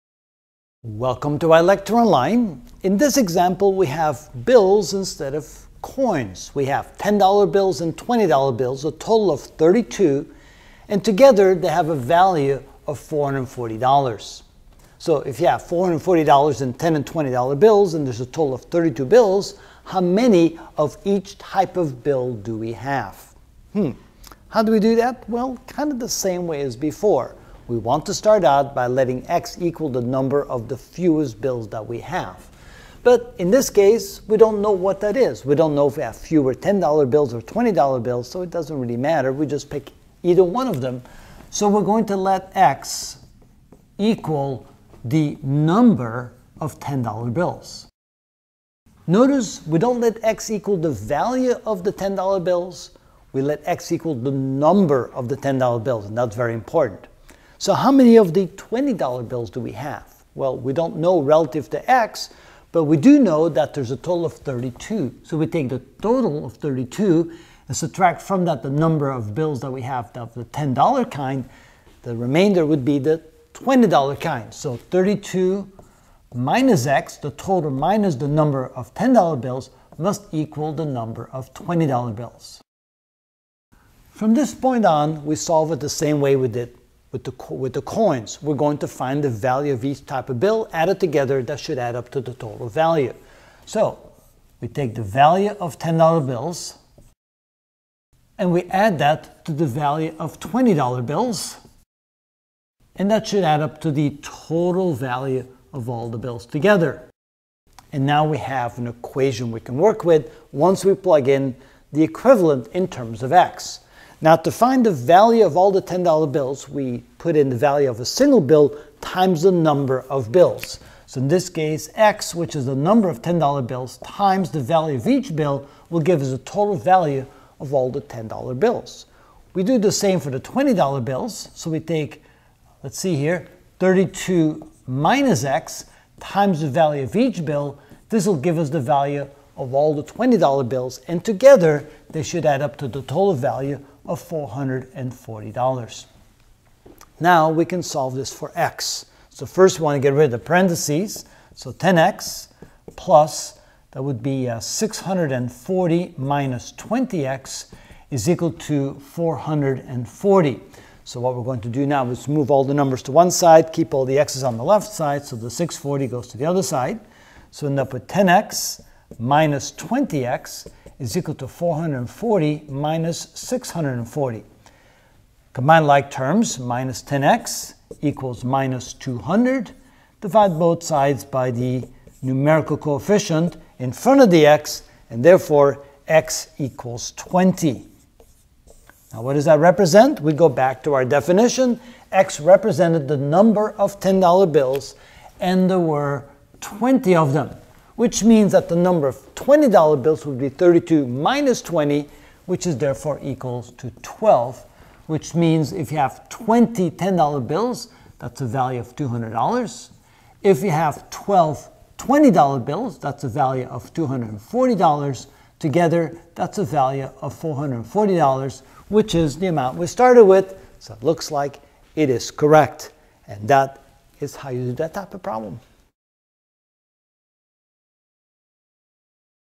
Algebra - Ch. 2: Word Problem (17 of 46) Type 5: Coins and Money: Ex. 3
In ths video I will solve Type 5 of Example 3 word problem: Andrew has $440 in $10 and $20 bills. He has 32 bills. How many of each type does he have?